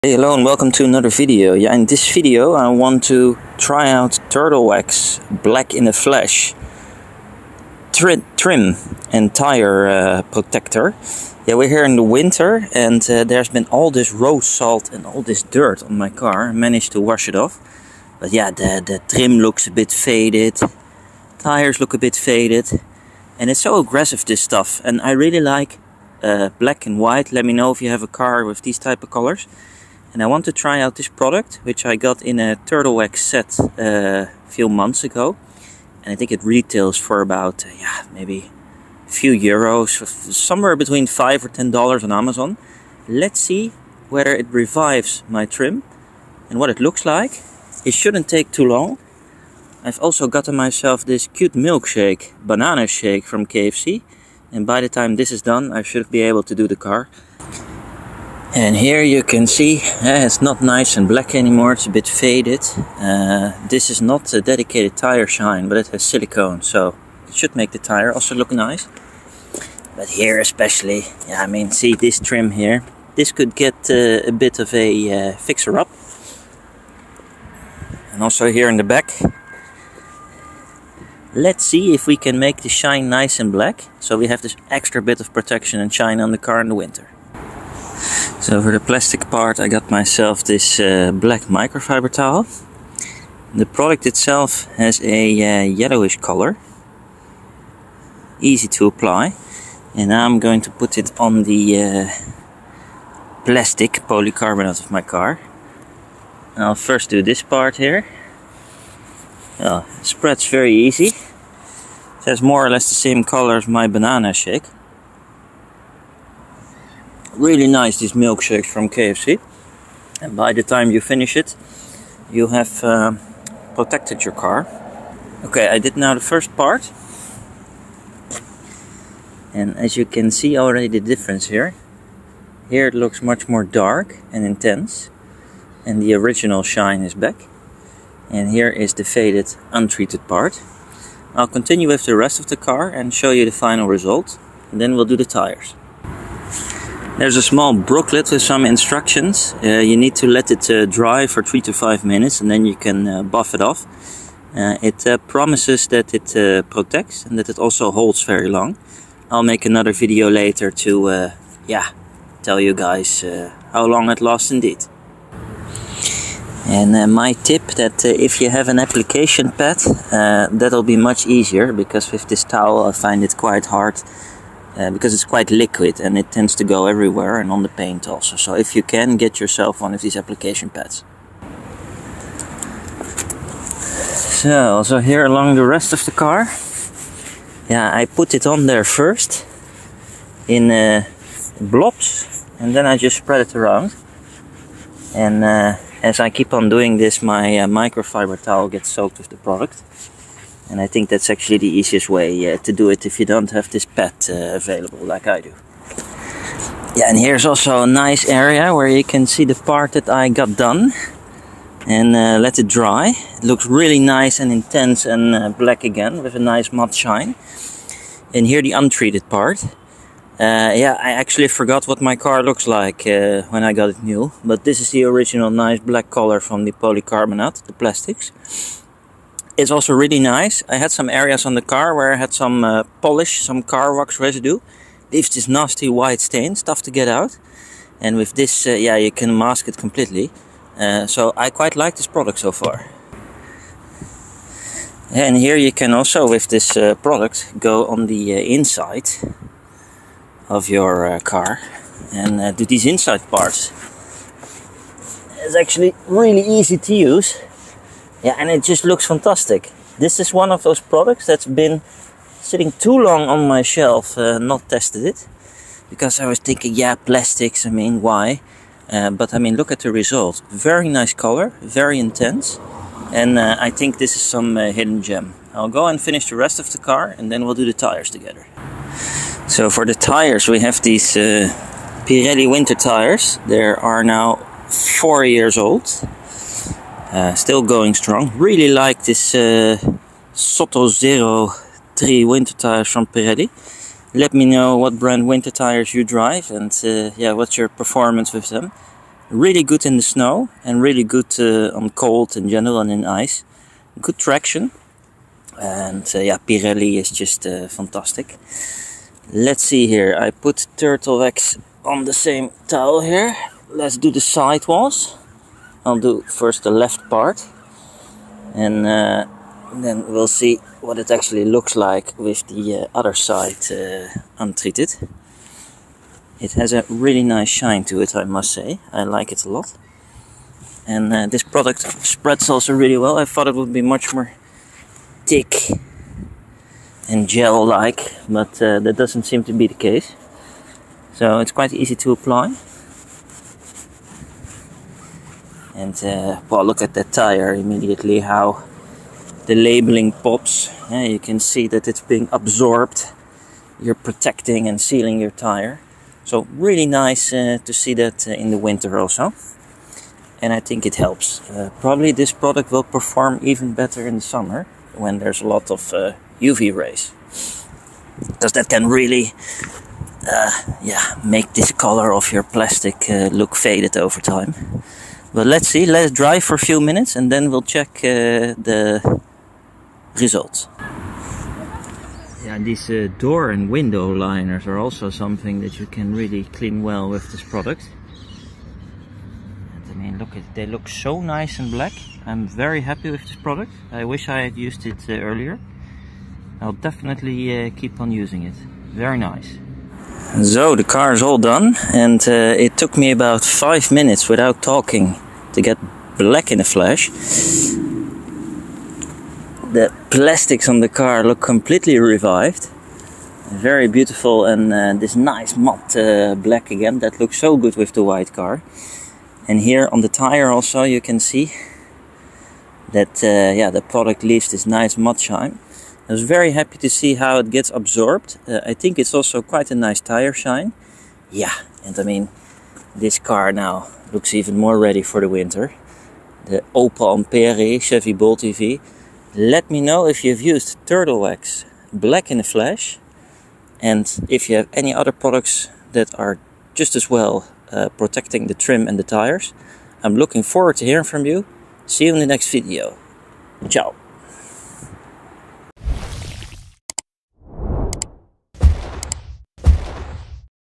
Winter tires & car trim protection - Black in a Flash by Turtle Wax review & test
The black trim and winter tires on my Opel Ampera-e / Chevy Bolt EV are in need of restoration and protection against all the winter road salt. In this video I will be trying out a new product I purchased called Turtle Wax Black in a Flash. It can be used on tires and plastic / Polycarbonate trim on the outside part of your car.

Find out how easy it is to apply and what it looks like on trim, door liners and tires. Here is a link on Amazon to the product I purchased myself:

Turtle Wax black in a flash

What's your experience with Turtle Wax Black in a Flash to protect your winter tires and car's trim? Is there another product you really like that you can recommend?